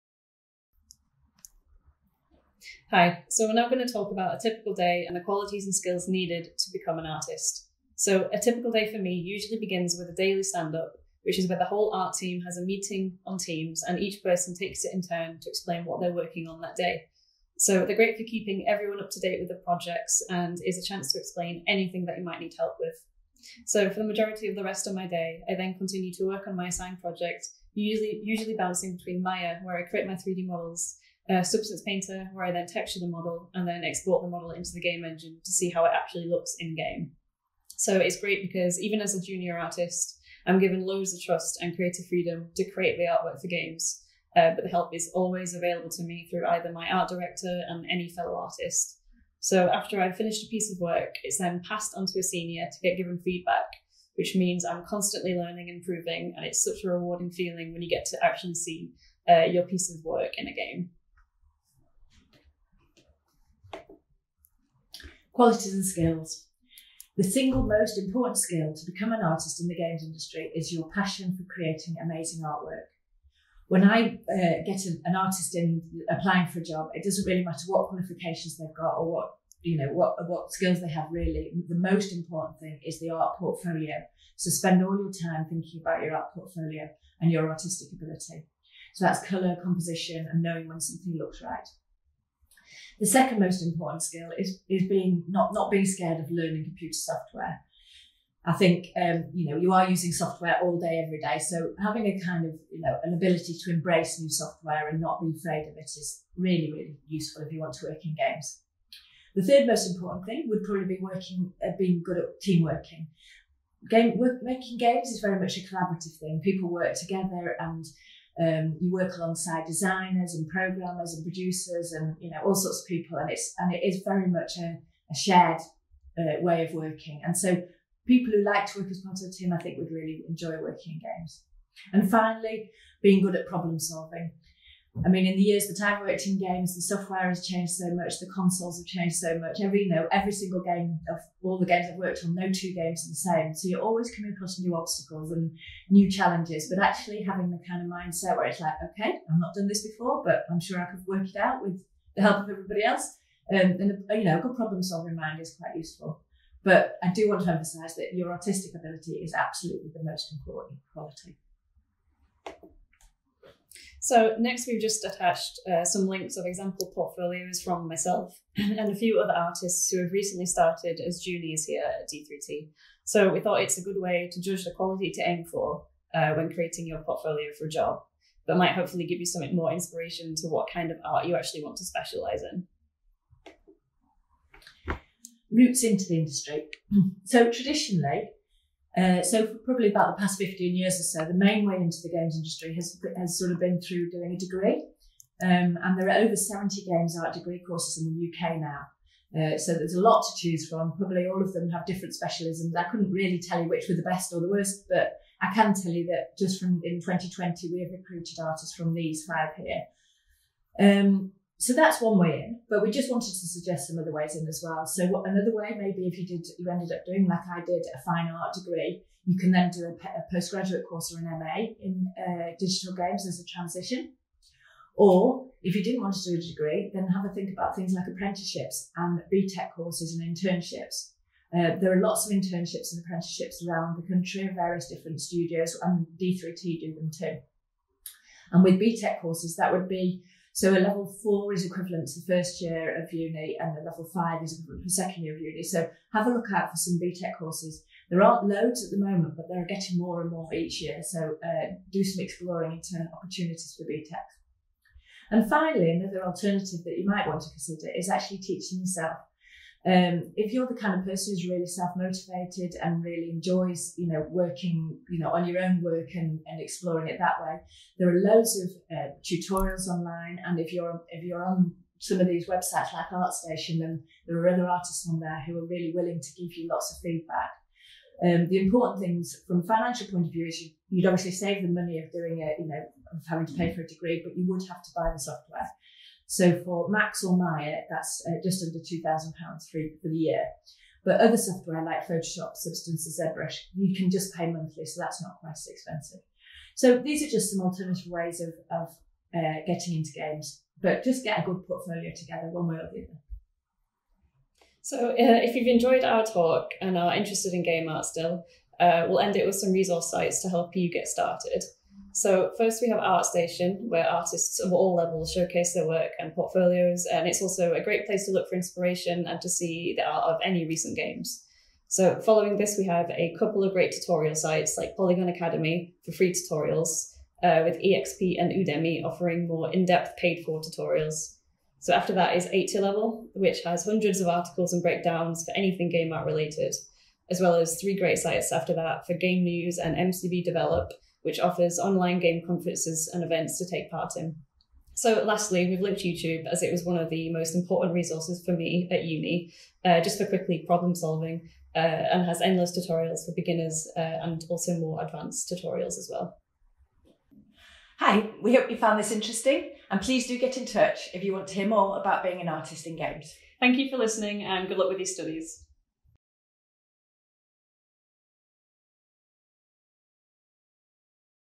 How to become a Games Artist with d3t - Part Four
We're joined by the wonderful folks at d3t to hear about the world of Games Art - and how you can prepare yourself for a career in it.

In Part Four, Art Manager Louise Andrew and Associate Artist Eleanor Biggin break down a typical day at d3t, as well as tips and tricks for making your games art portfolio shine to potential employers.